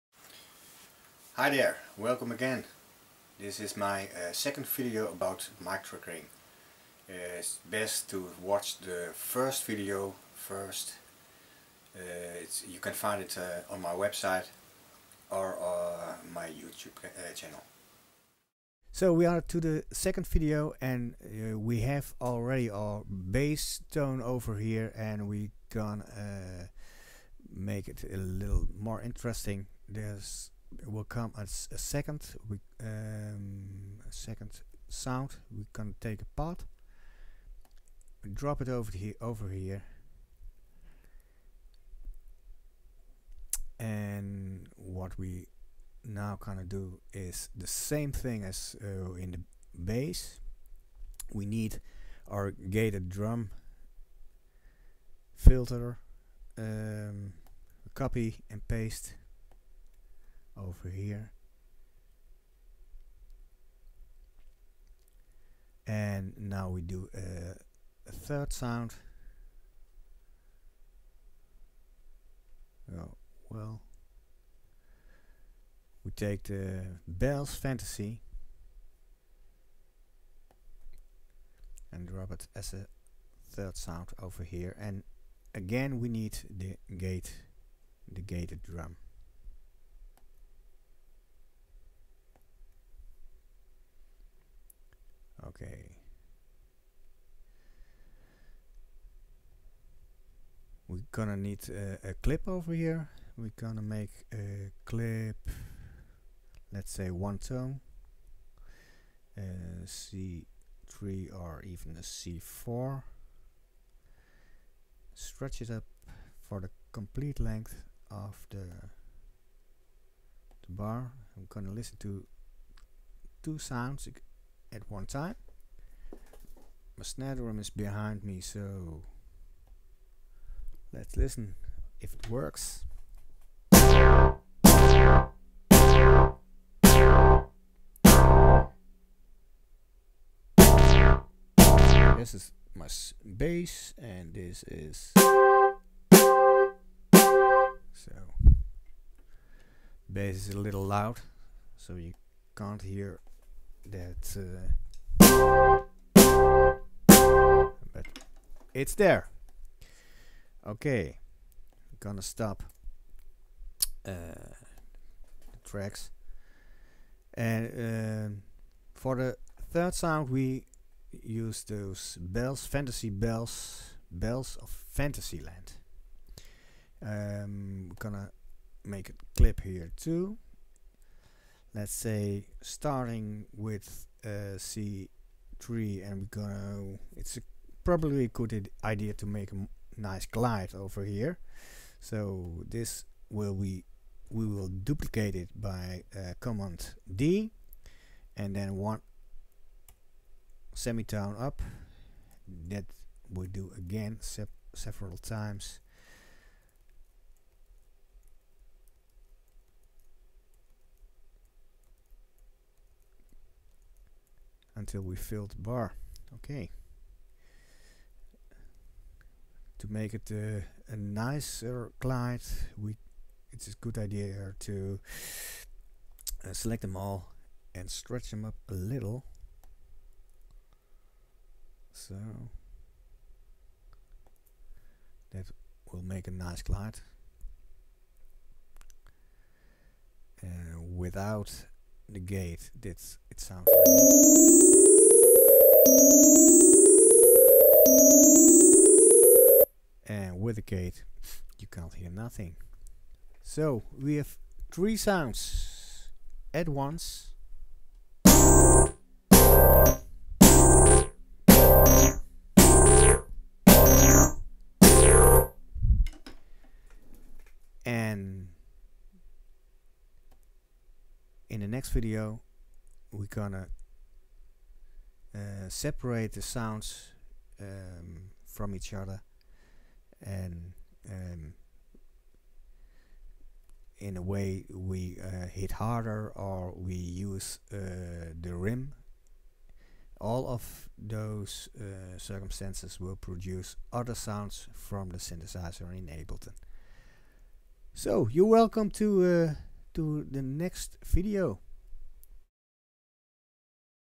Mic Triggering 2
How to trigger synthesizer sounds with a microphone in Ableton Live? This is the second of three video's in which it is explained.